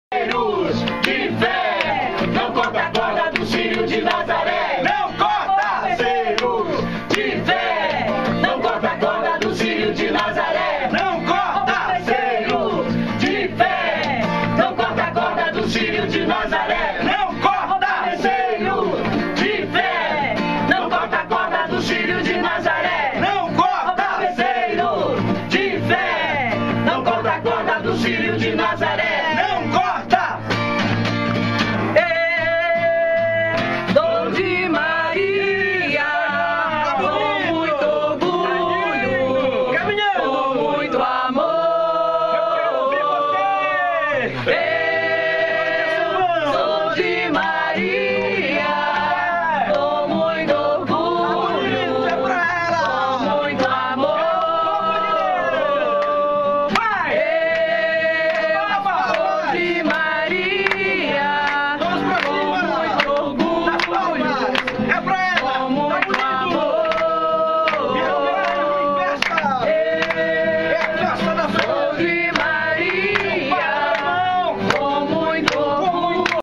Rádio Nazaré animando o Círio 2012
Animadores do Círio 2012 no estúdio da Rádio Nazaré